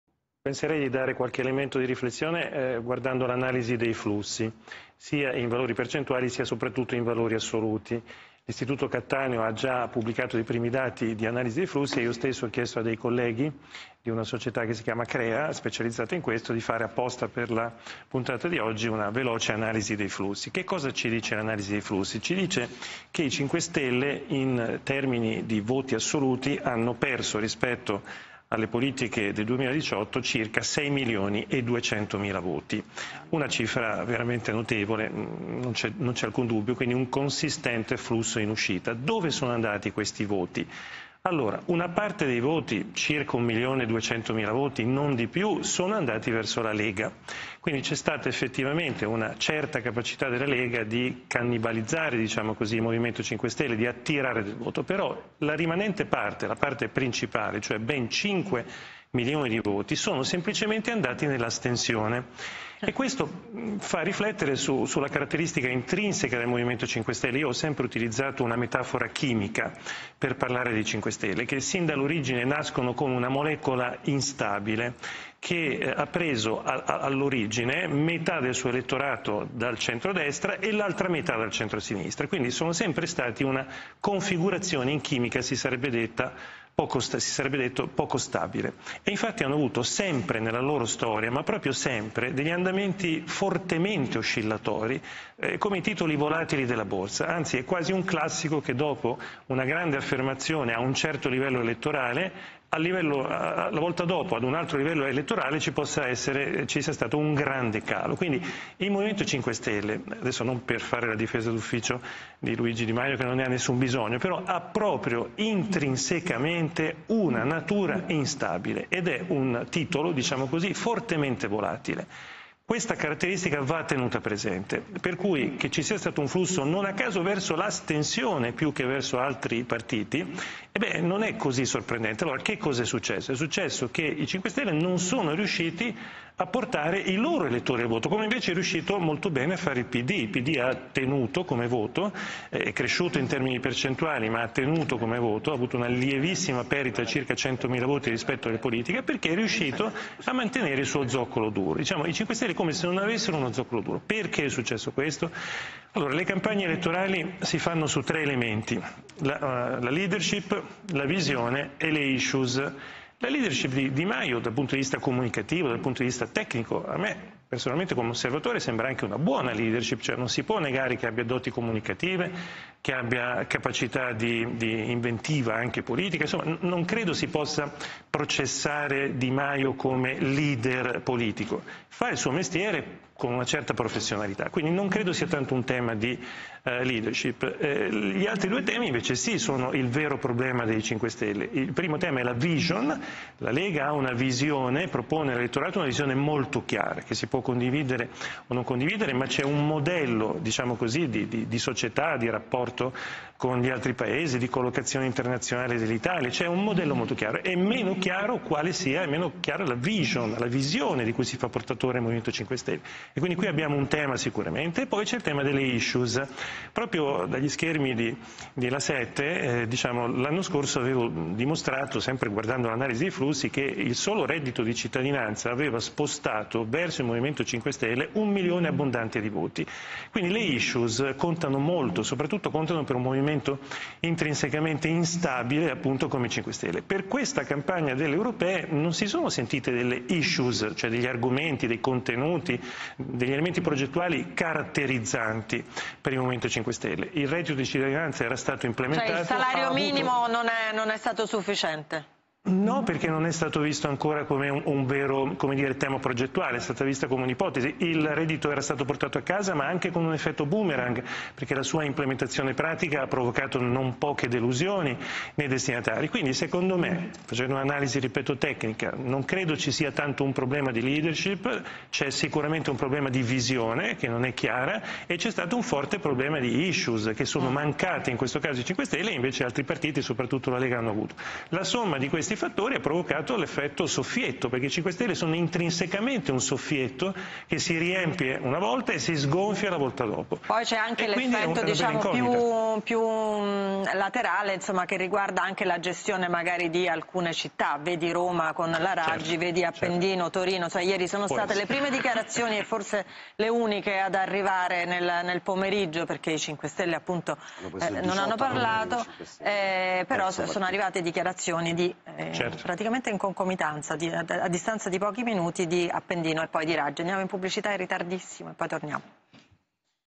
L'analisi dei flussi elettorali di Alessandro Amadori: 'I voti del M5S andati all'astensione, ...
Alessandro Amadori (Ist. Piepoli) analizza i flussi di voto del M5S alle scorse elezioni europee